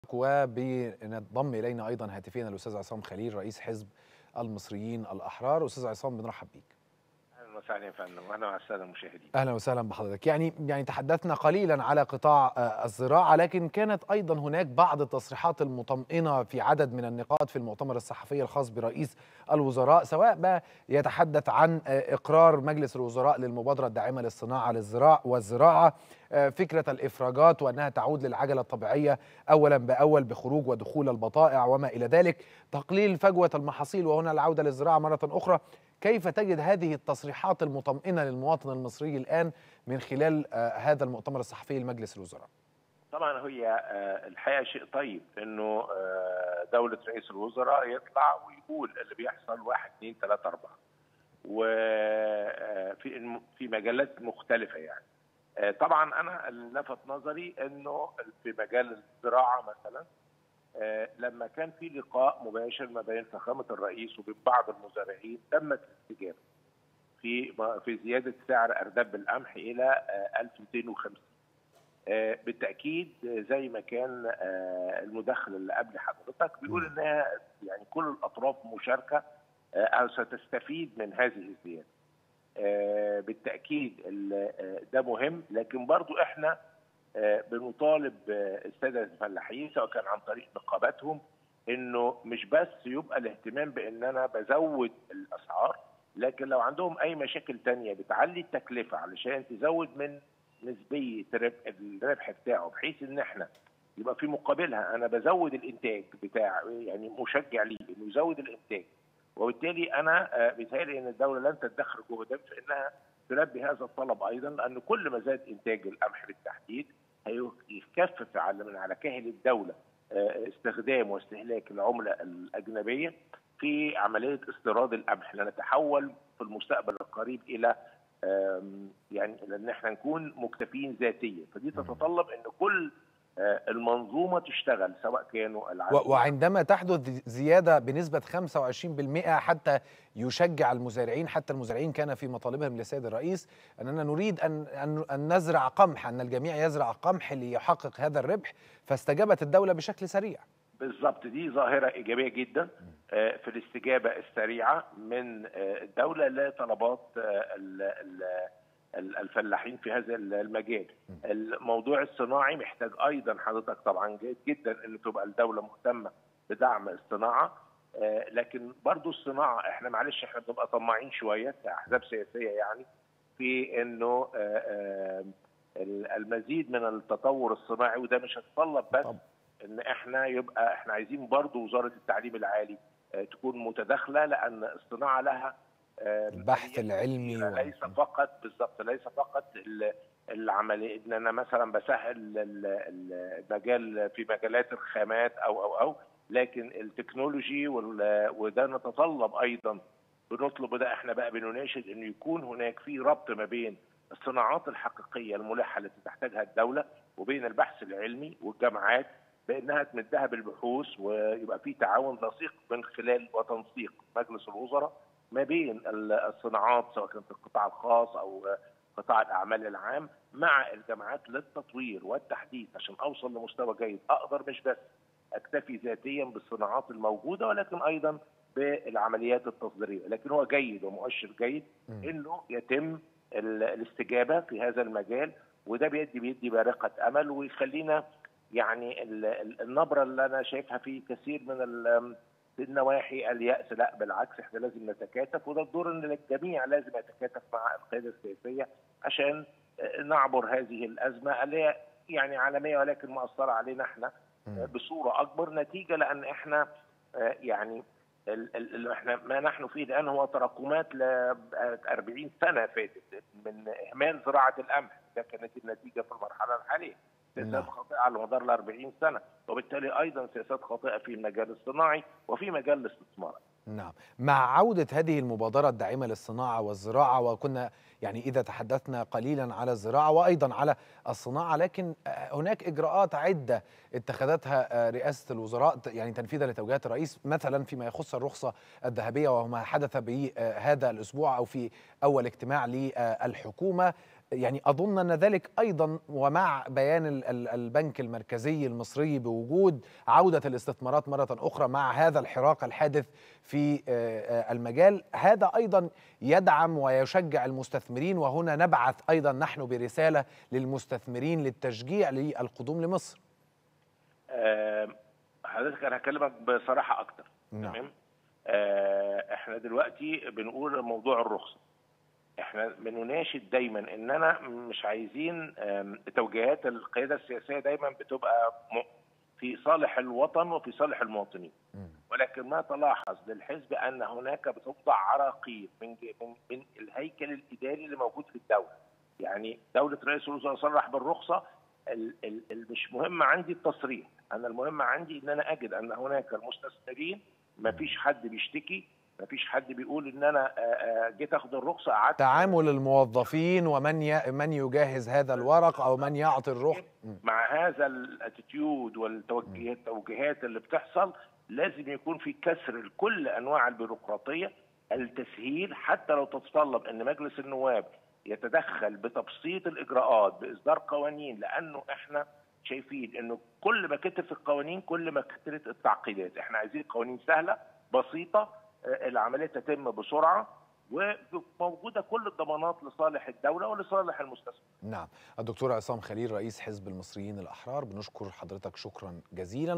عصام خليل رئيس حزب المصريين الأحرار: اجتماع رئاسة الوزراء يطمئن المواطنين
تغطية خاصة| عصام خليل رئيس حزب المصريين الأحرار: اجتماع رئاسة الوزراء يطمئن المواطنين

18 يناير 2023